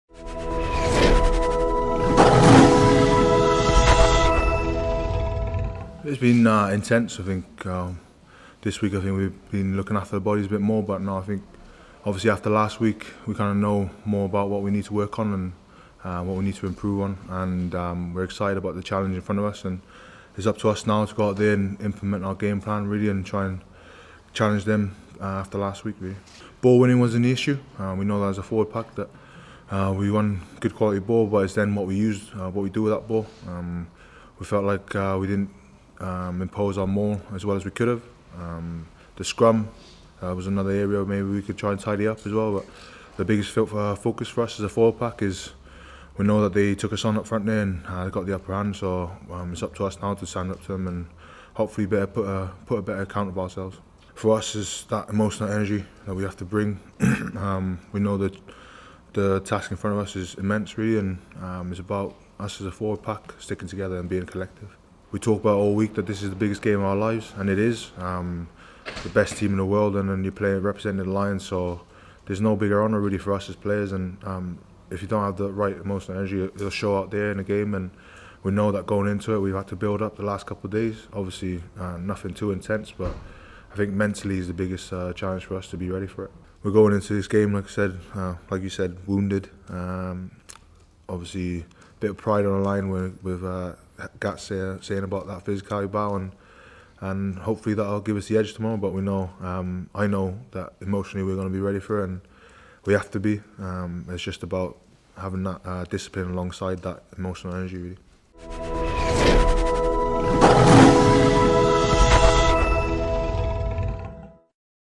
Press Conference: Vunipola backing Lions' emotional energy ahead of second Test | Lions NZ 2017
Mako Vunipola believes that emotional energy will be a key factor as to whether or not the British & Irish Lions can square the Test series at 1-1 in Wellington this Saturday.